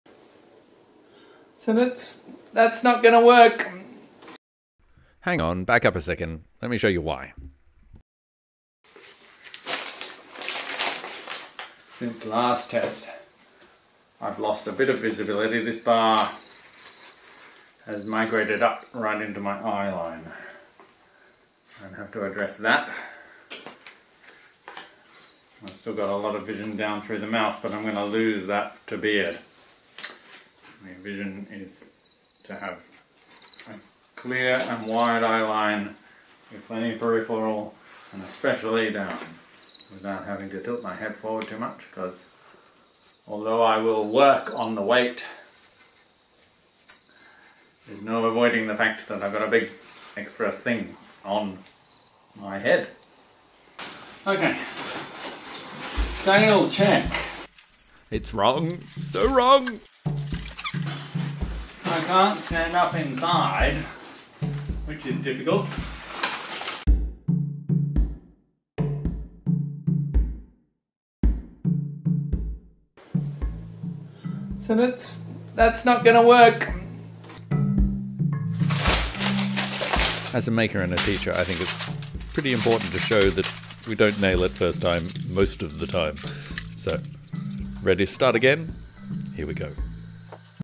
Ent Costume Pt. 8 Dismantling a head
In this episode of our Ent costume making series, I dismantling an unsatisfactory head for my stilts-based costume. I also discuss the reasons behind the decision to take apart the head.